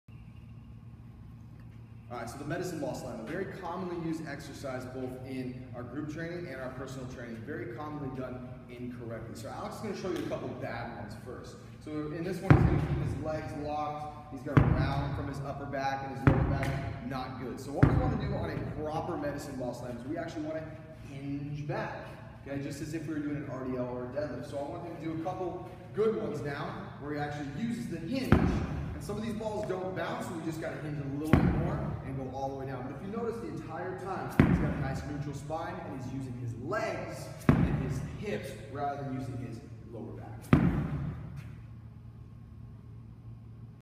Proper Medicine Ball Slam Technique
One of the most commonly used exercises in strength and conditioning programs with any goal is the medicine ball slam. Done with light weight and lower reps, it is fantastic for power production. Done with heavier weight and more reps, it is amazing for cardio/conditioning. However, in either case, proper form is a must, and unfortunately this exercise is performed incorrectly more than it isn't. Much like a kettlebell swing, the medicine ball slam is ACTUALLY a hinge pattern rather than a squat pattern. So instead of rounding over by flexing the spine, with soft knees, neutral spine, and braced core, slam the ball down by driving the hips back as if trying to touch an imaginary wall behind you. Your lower back will thank you!

When you're ready to get stronger, look better, and feel good again here are a few ways we can help.